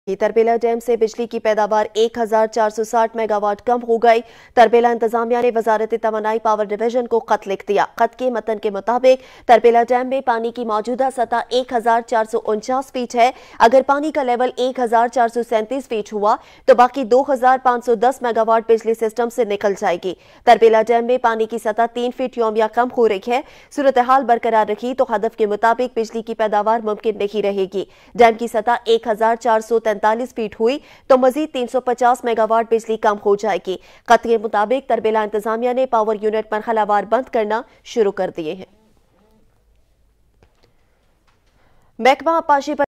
Power generation decreased, how much Mega Watt??
After the gas shortfall, there will be water shortage again. The power generation from Tarbela Dam has been reduced by 1460 MW. The Tarbela administration has written a letter to the Ministry of Energy and Power Division.